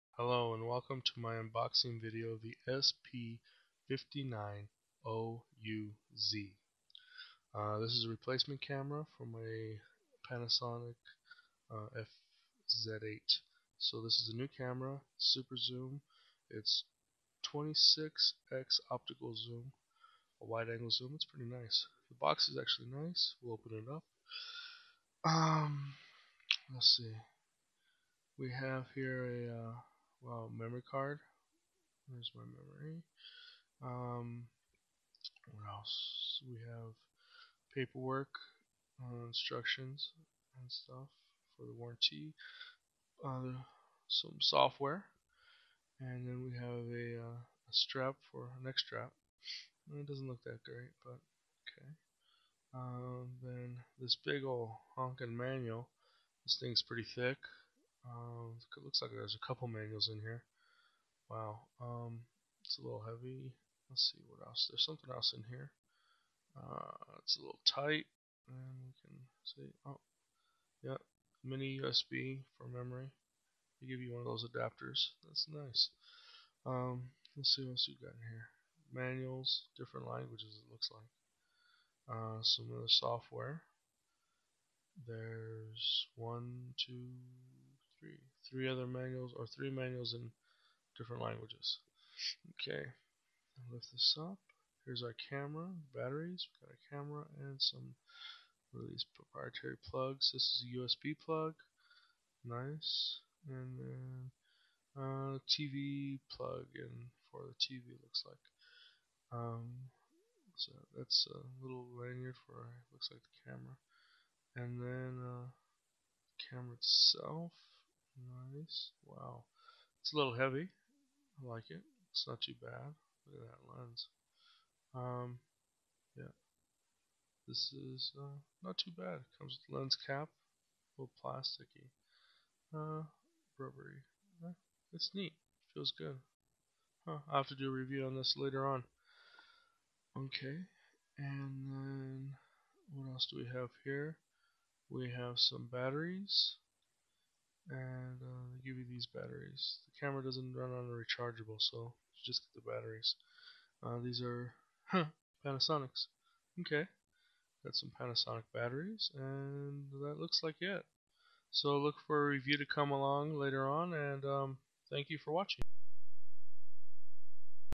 opening The Olympus SP-590UZ
my new Olympus SP-590UZ  with 26x Optical Zoom + 5x Digital Zoom